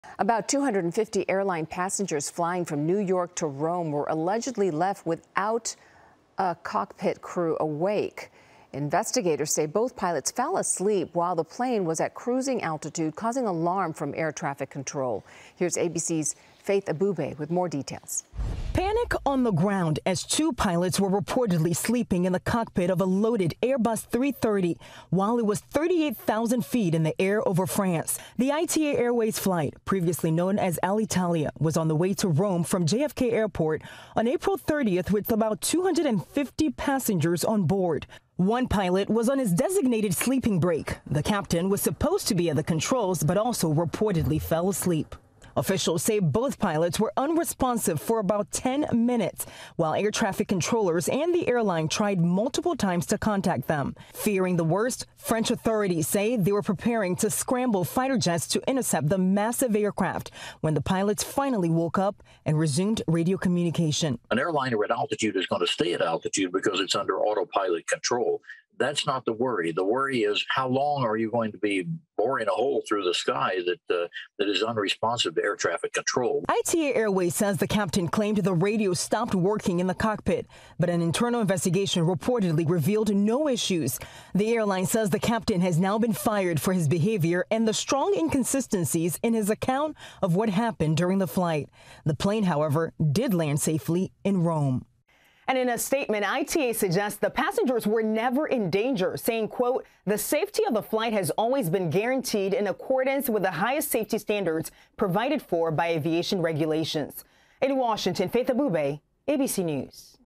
2 pilots allegedly fall asleep on flight from New York to Rome; 250 passengers on board | ABC7
Two airline pilots reportedly fell asleep on a flight from New York City to Rome last month while their plane was still in the air with as many as 250 passengers on board, investigators found. MORE